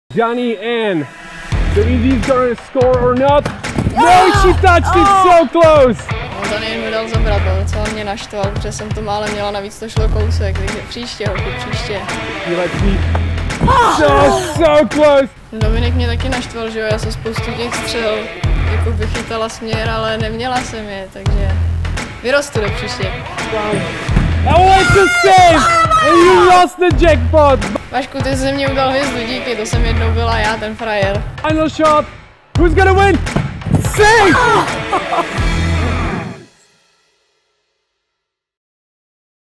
VIP Shootout with Alexandra Vanickova [Highlights]
Our first female Live Penalty VIP Shootout. Not only did we have Alexandra Vanickova from AC Sparta Prague on stage, but also a special VIP guest who shot some balls and scored some goals against her. Victoire Cogevina – co-founder of Gloria app – flew in all the way from San Francisco to join this special ladies’ evening.

Here are some of the highlights.

Download and play against real goalkeepers in our LIVE PENALTY game!